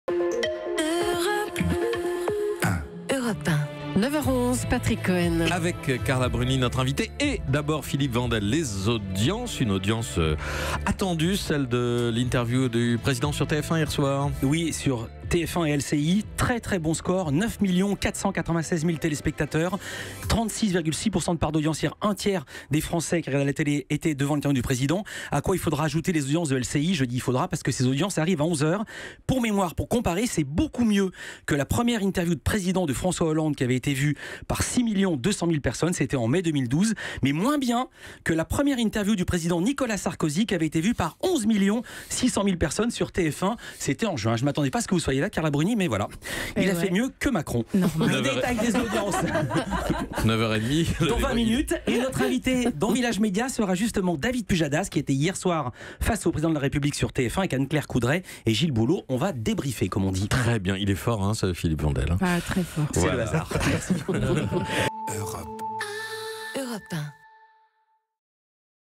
Emmanuel Macron sur TF1 : un tiers des Français ont regardé la première interview du président
Chaque jour, Philippe Vandel fait le point sur un élément précis des audiences télé de la veille.